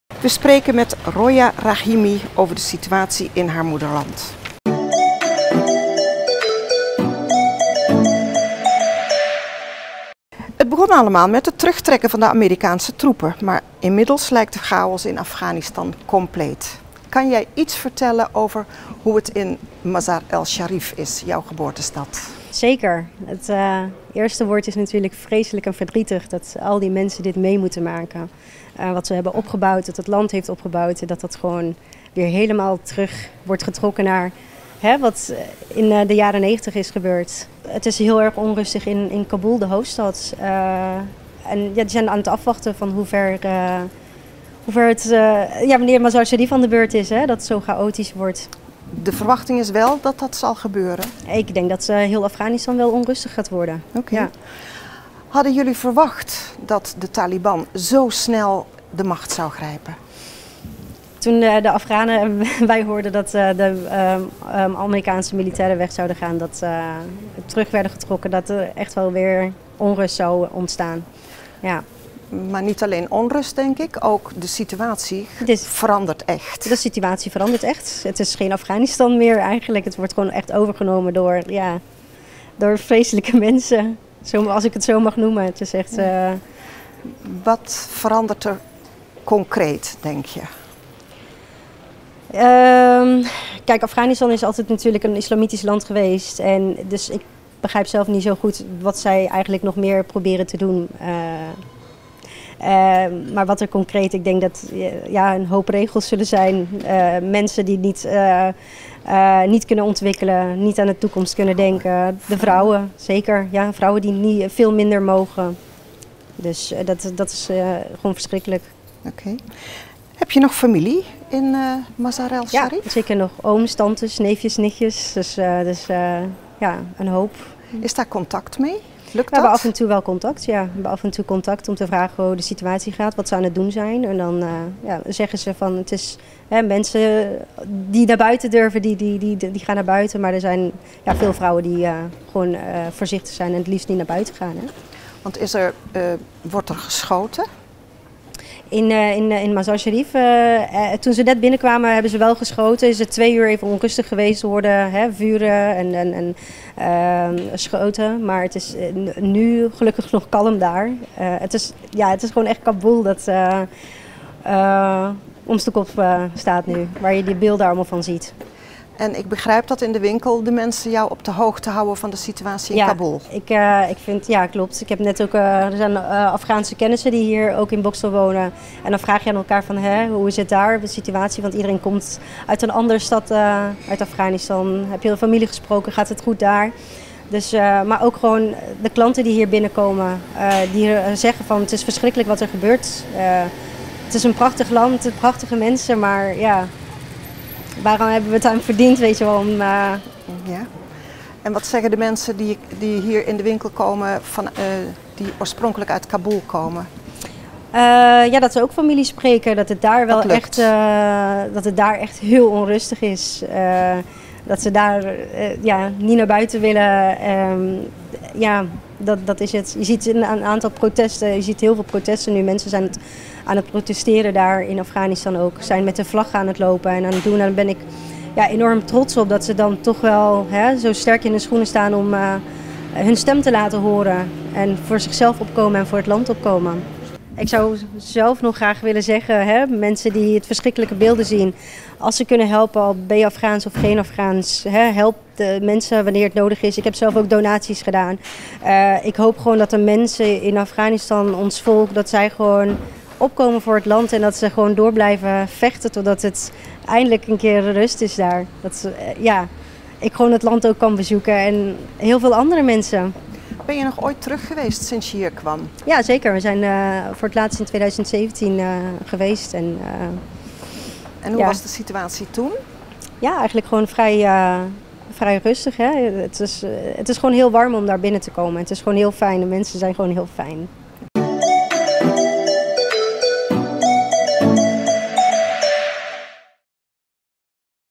Interview met Roya Rahimi
In augustus 2021 heeft de Taliban de macht over het land gegrepen en is de democratisch gekozen regering gevlucht